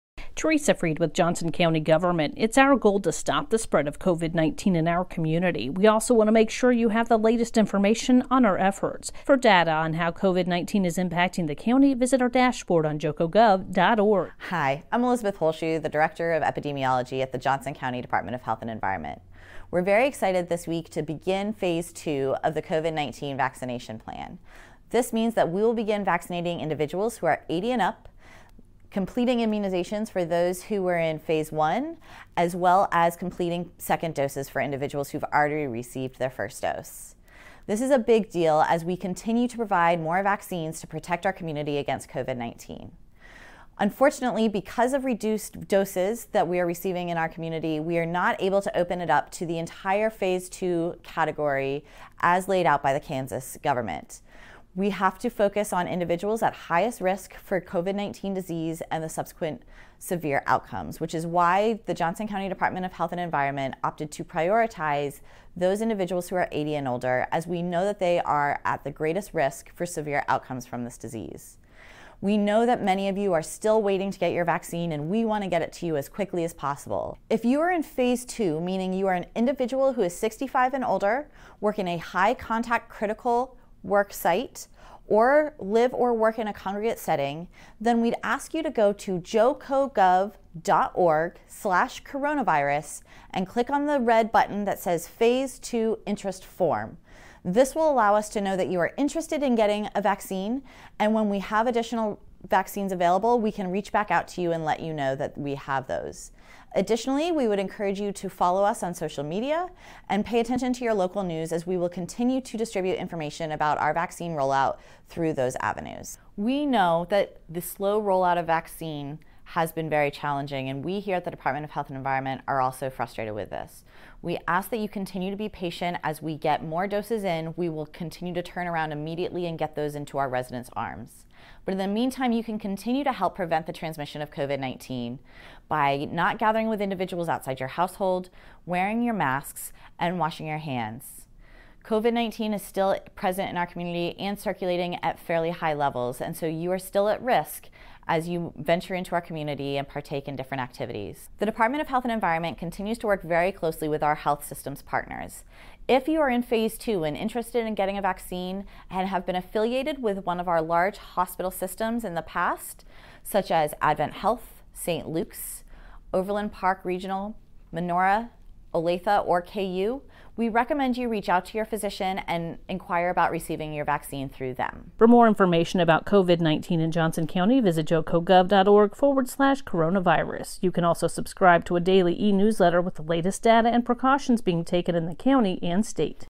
Johnson County COVID-19 Weekly Update 1.25.21
Johnson County Department of Health and Environment Epidemiology Director Elizabeth Holzschuh discusses the county's move to Phase 2, Tier 1 of the COVID-19 vaccination rollout plan.